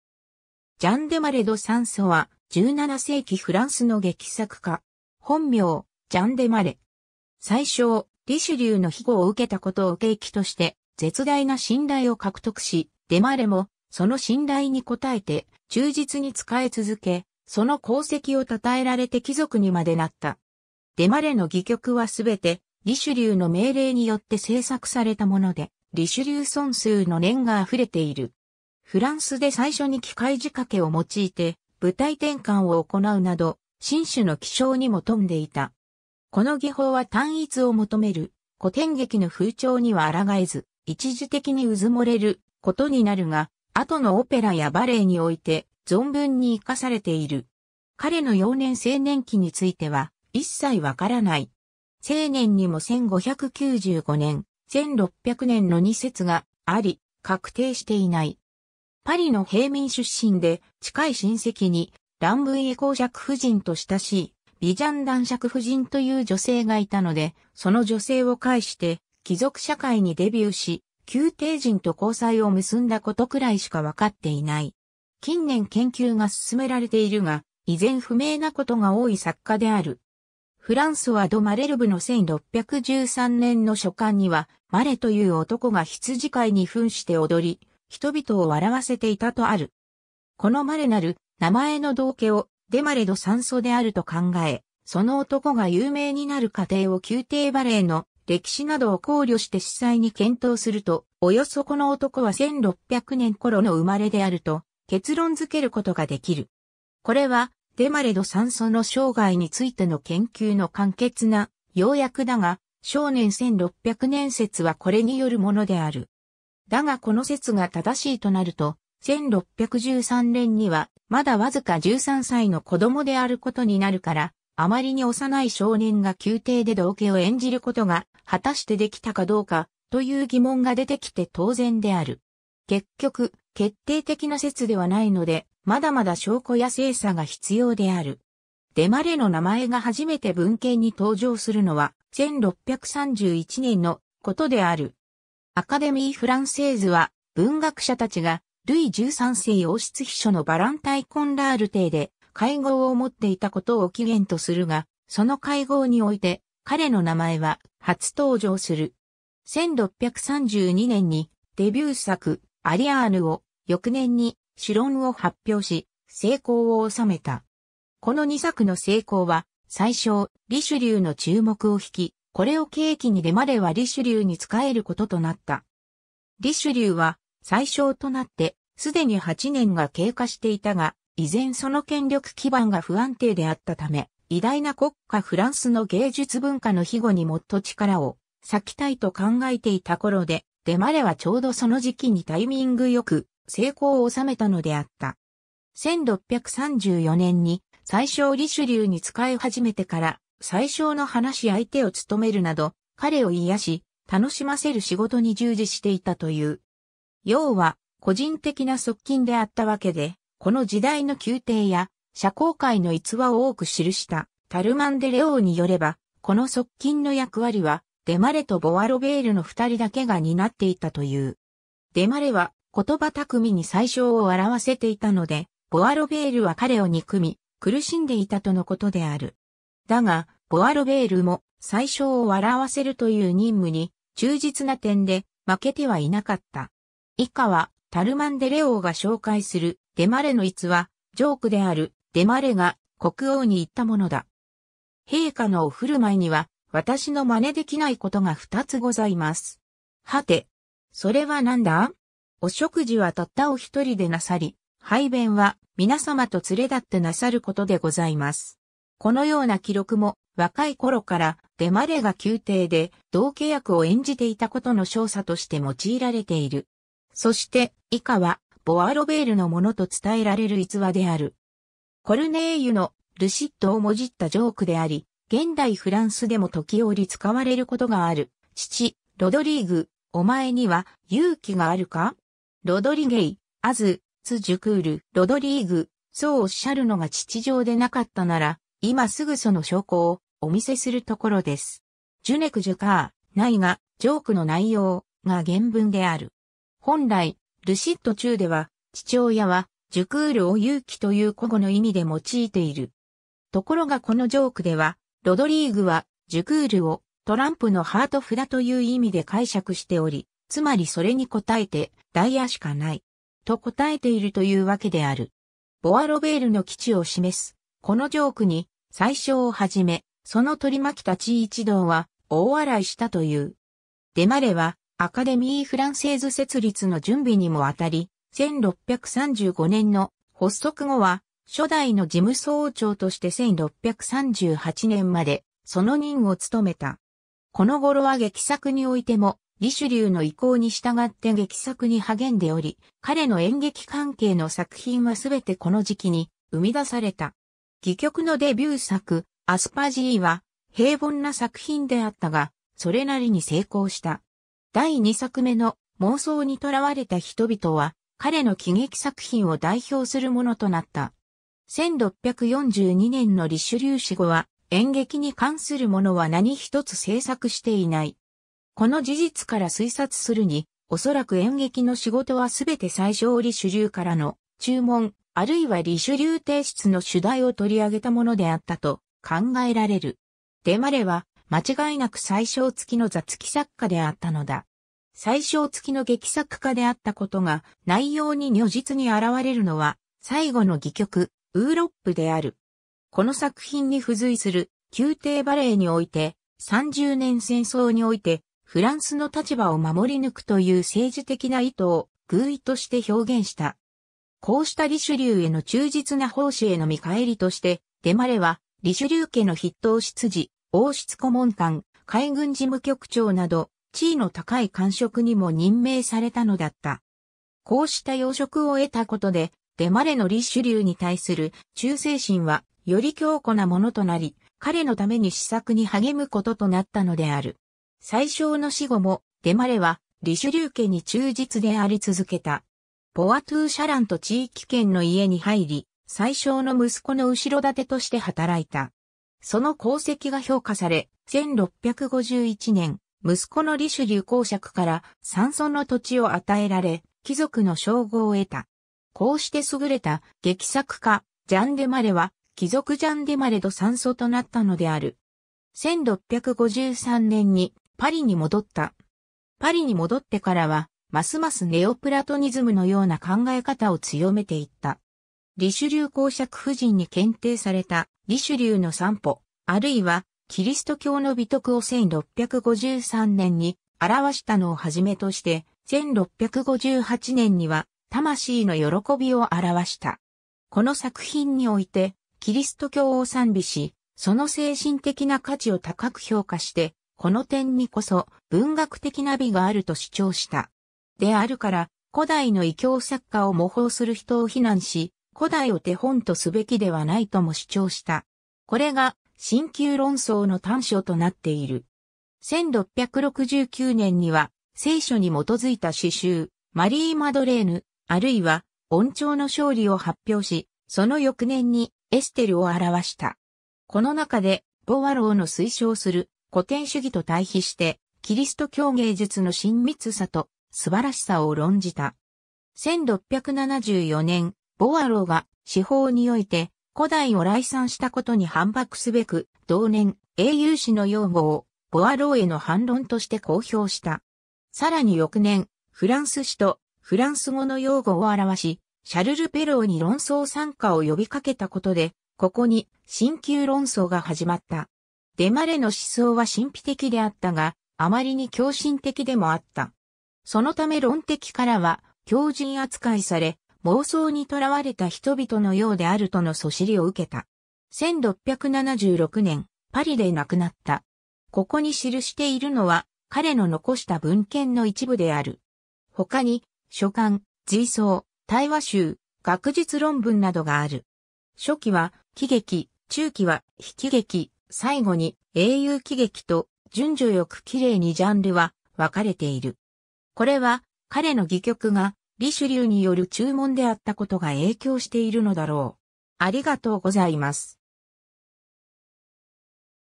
ジャン・デマレ・ド・サン＝ソルラン
ジャン・デマレ・ド・サン=ソルラン（仏: Jean Desmarets de Saint-Sorlin 、1595年、1600年 - 1676年10月28日）は、17世紀フランスの劇作家。
本名:ジャン・デマレ。
宰相リシュリューの庇護を受けたことを契機として絶大な信頼を獲得し、デマレもその信頼に応えて忠実に仕え続け、その功績を讃えられて貴族にまでなった。
デマレの戯曲はすべてリシュリューの命令によって製作されたもので、リシュリュー尊崇の念が溢れている。
フランスで最初に機械仕掛けを用いて舞台転換を行うなど、進取の気性にも富んでいた。
この技法は単一を求める古典劇の風潮には抗えず、一時的に埋もれることになるが、後のオペラやバレエにおいて存分に活かされている。
 彼の幼年～青年期については、一切わからない。
生年にも1595年、1600年の2説があり、確定していない。
パリの平民出身で、近い親戚にランブイエ侯爵夫人と親しい、ヴィジャン男爵夫人という女性がいたので、その女性を介して貴族社会にデビューし、宮廷人と交際を結んだことくらいしか分かっていない。
近年研究が進められているが、依然不明なことが多い作家である。
 フランソワ・ド・マレルブの1613年の書簡には、「マレ(Maret)という男が羊飼いに扮して踊り、人々を笑わせていた」とある。
このマレなる名前の道化をデマレ・ド・サン＝ソルランであると考え、その男が有名になる過程を宮廷バレエの歴史などを考慮して仔細に検討すると、およそこの男は1600年ころの生まれであると結論付けることができる。
これはデマレ・ド・サン＝ソルランの生涯についての研究の簡潔な要約だが、生年1600年説はこれによるものである。
だがこの説が正しいとなると、1613年にはまだわずか13歳の子供であることになるから、あまりに幼い少年が宮廷で道化を演じることが果たして出来たかどうか、という疑問が出てきて当然である。
結局、決定的な説ではないので、まだまだ証拠や精査が必要である。
 デマレの名前が初めて文献に登場するのは、1631年のことである。
アカデミー・フランセーズは、文学者たちがルイ13世王室秘書のヴァランタン・コンラール邸で会合を持っていたことを起源とするが、その会合において彼の名前は初登場する。
1632年にデビュー作『アリアーヌ』を、翌年に『詩論』を発表し、成功を収めた。
この2作の成功は宰相リシュリューの注目を惹き、これを契機にデマレはリシュリューに仕えることとなった。
リシュリューは宰相となってすでに8年が経過していたが、依然その権力基盤が不安定であったため、偉大な国家フランスの芸術文化の庇護にもっと力を割きたいと考えていた頃で、デマレはちょうどその時期にタイミングよく成功を収めたのであった。
 1634年に宰相リシュリューに仕え始めてから、宰相の話し相手を務めるなど、彼を癒し、楽しませる仕事に従事していたという。
要は個人的な側近であったわけで、この時代の宮廷や社交界の逸話を多く記したタルマン・デ・レオーによれば、この側近の役割はデマレとボワロベールの2人だけが担っていたという。
デマレは言葉巧みに宰相を笑わせていたので、ボワロベールは彼を憎み、苦しんでいたとのことである。
だが、ボワロベールも宰相を笑わせるという任務に忠実な点で、負けてはいなかった。
以下はタルマン・デ・レオーが紹介するデマレの逸話、ジョークである: デマレが国王に言ったものだ。
「陛下のお振舞いには、私の真似できないことが2つございます。」「はて！それはなんだ？」「お食事はたったお1人でなさり、排便は皆様と連れ立ってなさることでございます。」 このような記録も、若いころからデマレが宮廷で道化役を演じていたことの証左として用いられている。
そして、以下はボワロベールのものと伝えられる逸話である。
コルネイユの『ル・シッド』をもじったジョークであり、現代フランスでも時折使われることがある: 父:ロドリーグ、お前には勇気(ハート)があるか？ 「Rodrigue,as-tu du coeur?」 ロドリーグ:そう仰るのが父上でなかったなら、今すぐその証拠をお見せするところです。
(ダイヤしかないな) 「Je n'ay que du carreau」 ()内がジョークの内容、「」が原文である。
本来『ル・シッド』中...